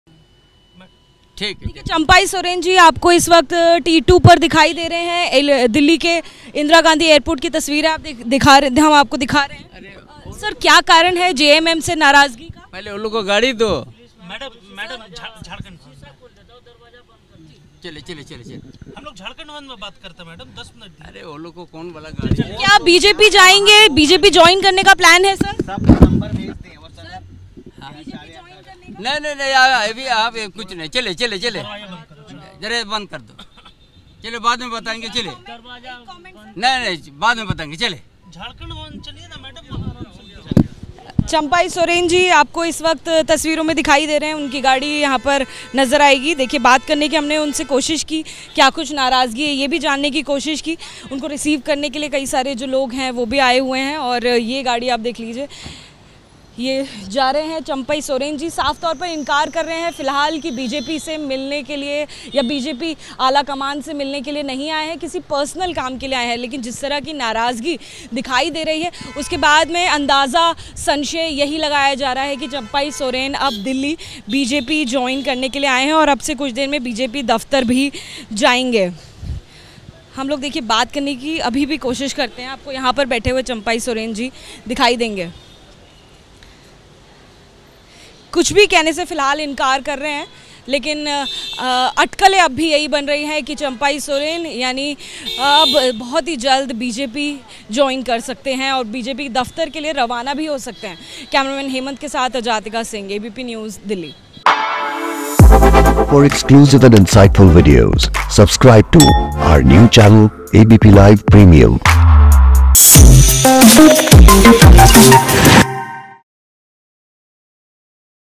Jharkhand को लेकर फिर हलचल बढ़ी, Champai Soren एक बार फिर दिल्ली पहुंचे
ABP News: झारखंड को लेकर फिर हलचल बढ़ी, चंपाई सोरेन एक बार फिर दिल्ली पहुंचे
abp न्यूज़ से बोले चंपाई- जल्द अगली रणनीति बताऊंगा